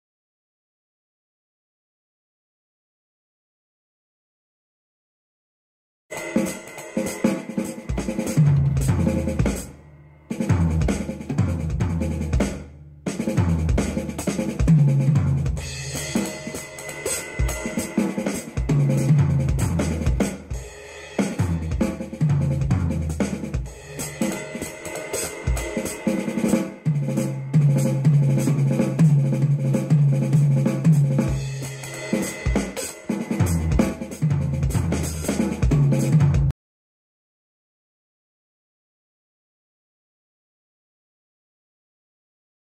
Working with 5's + 6's - Andy Ferguson Drums
Drums, Rudiments, Drum Fills, Jazz, Swing, Applying some 5 and 6 stroke stickings around the kit ..slipping a kick here and there.